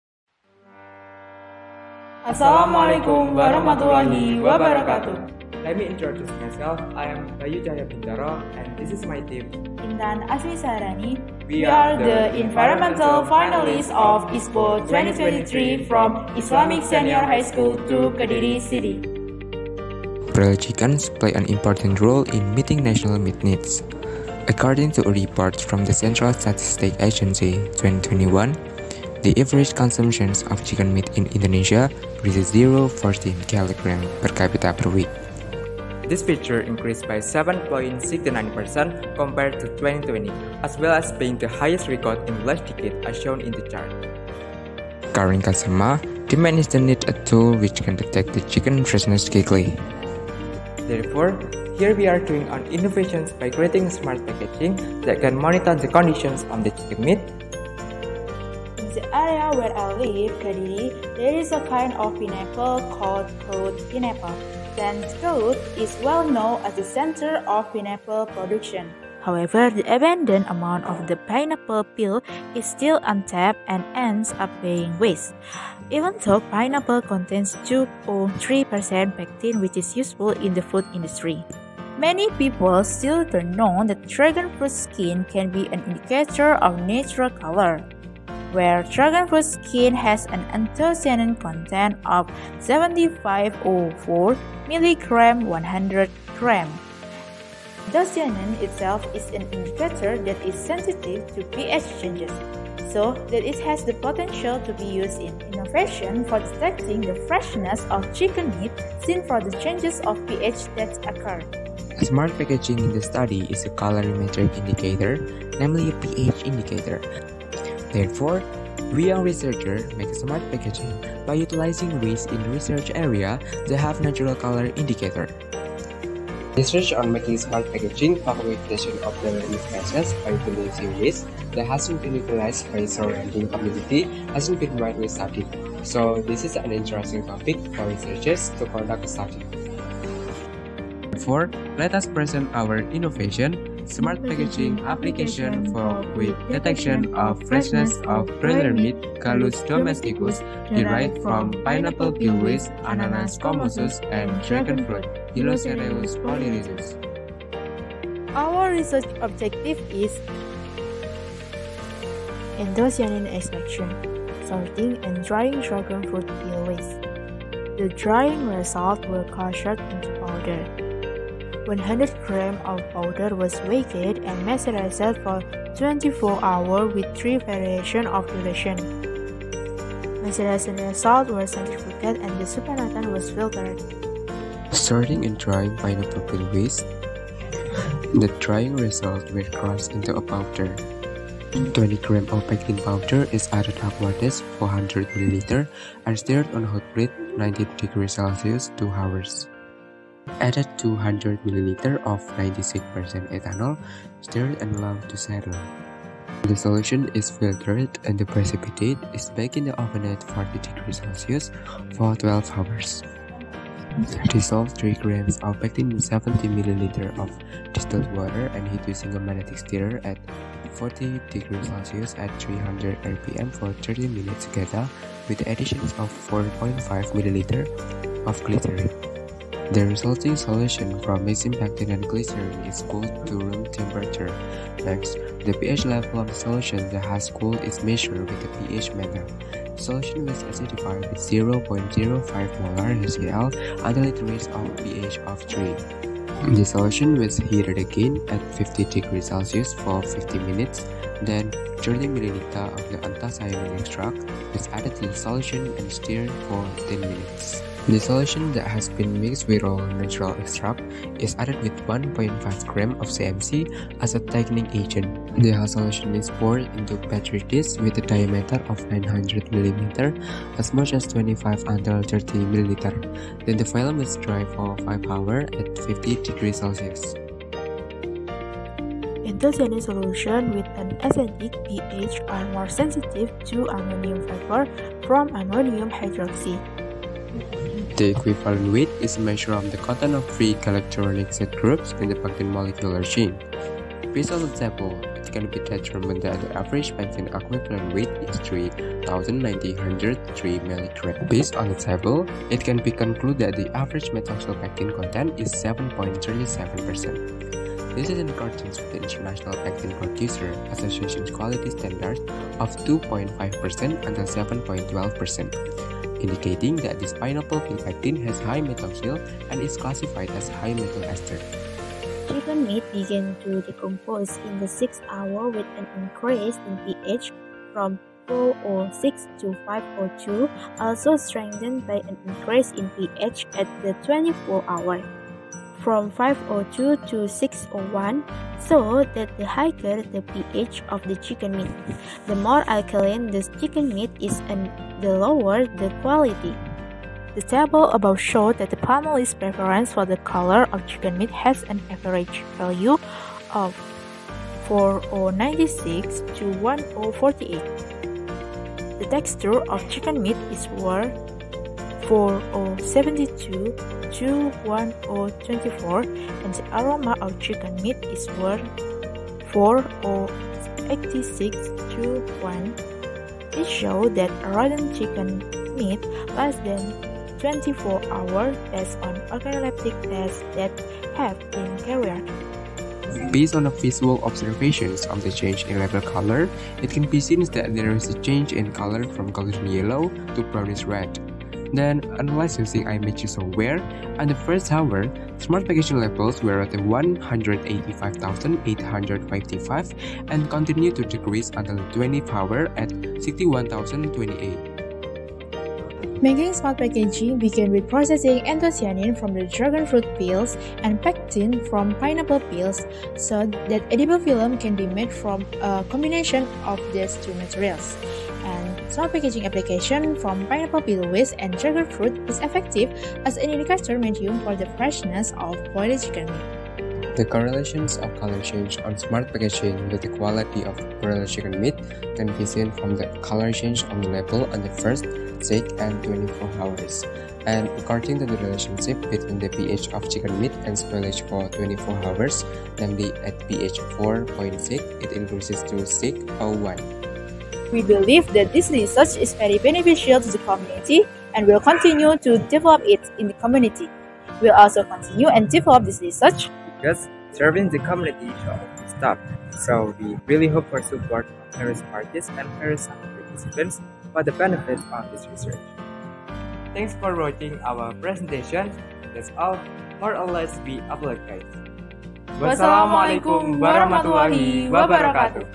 KIR An-Nahl || Finalis ISPO 2023 Man 2 Kota Kediri
Nama peneliti 1 :
Bayu Cahyo Bintoro
Nama peneliti 2 :
Intan Asmi Saharani
Judul Penelitian :
Smart Packaging Application for Quick Detection of
Freshness of Broiler Meat (Gallus domesticus) Derived on
Pineapple Peel Waste (Ananas comosus) and Dragon Fruit
(Hylocereus polyrhizus)
Asal sekolah :
MAN 2 Kota Kediri
Nama pembimbing :
1. Rr. Dewi MKA, S. Pd.
2. Ahya Mujahidin M. Pd.

Smart packaging merupakan inovasi label pendeteksi berbahan dasar limbah kulit nanas dan kulit buah naga guna memonitor kesegaran daging ayam broiler. Label pendeteksi diletakkan di kemasan daging ayam dan akan menunjukkan perubahan warna label dari kuning keemasan menjadi coklat kemerahan. hal ini mengindikasikan kualitas mutu daging yang semakin menurun.